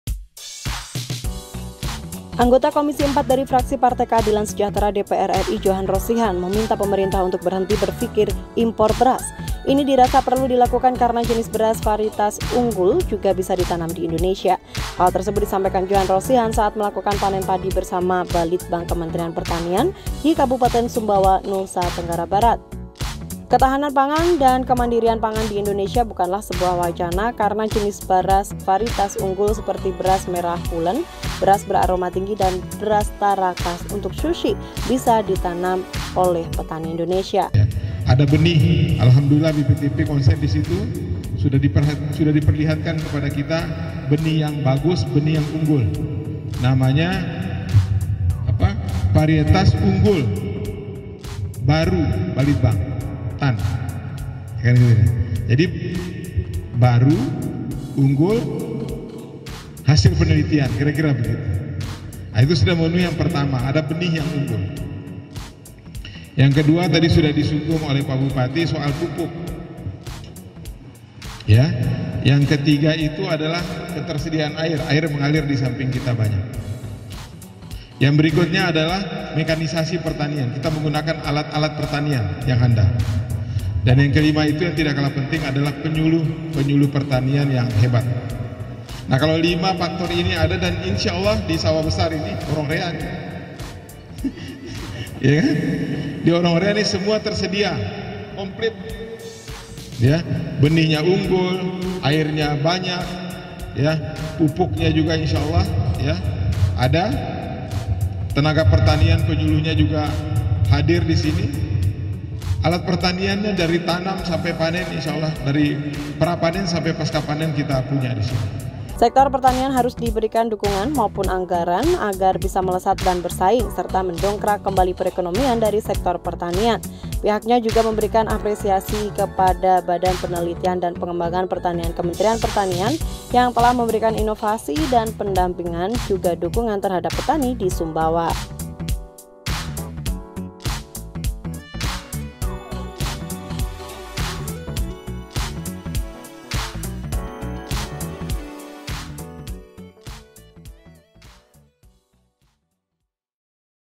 Johan Rosihan: Berhenti Berpikir Impor Beras
Anggota Komisi IV DPR RI Johan Rosihan menyampaikan agar Indonesia berhenti berpikir untuk impor beras. Indonesia dirasa mampu memproduksi beras varietas unggul.